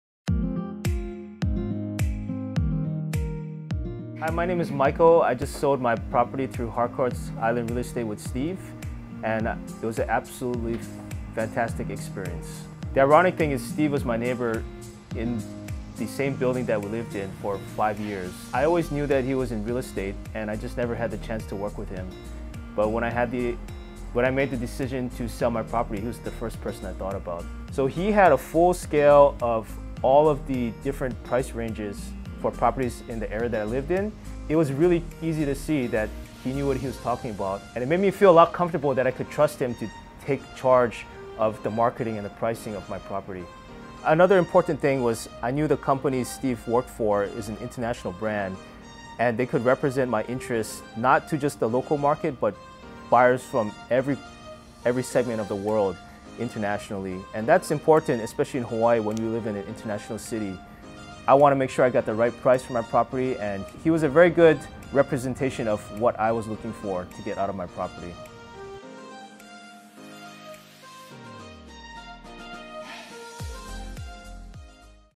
Client Testimonial - Michael Chan
Michael Chan describes his experience in working with Steve Fierros of Harcourts Island Real Estate on the sale of his Pacifica Honolulu condo in Kakaako.

Interested in Buying or Selling a Home 🏡 or Condo 🏢 in Hawaii 🏝️? We'd love to help 🤙